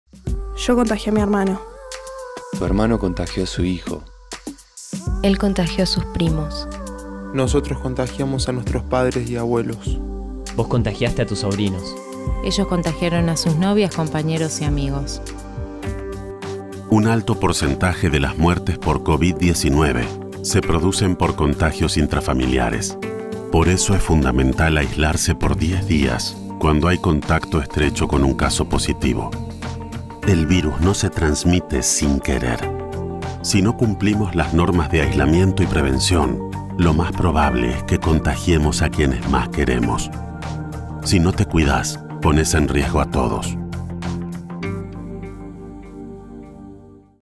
Contacto Intrafamiliar
Aislate por 10 días si estuviste en contacto estrecho con un caso positivo. No pongas en riesgo a los que más querés.